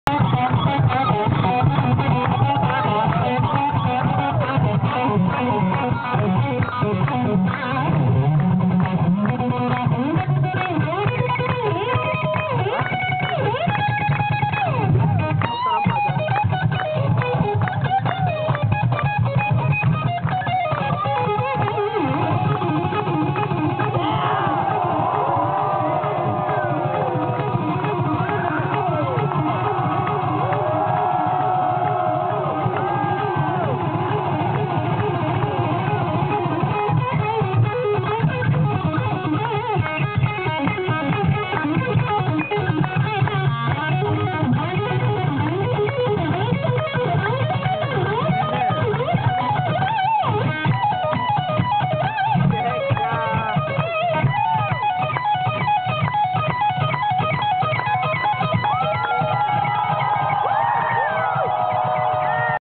MR.BIG Reunion World Tour 2009 18th October@Fort Canning,Singapore! Paul Gilbert Solo
MR.BIG Reunion World Tour 2009 18th October@Fort Canning,Singapore! Paul Gilbert fools around abit...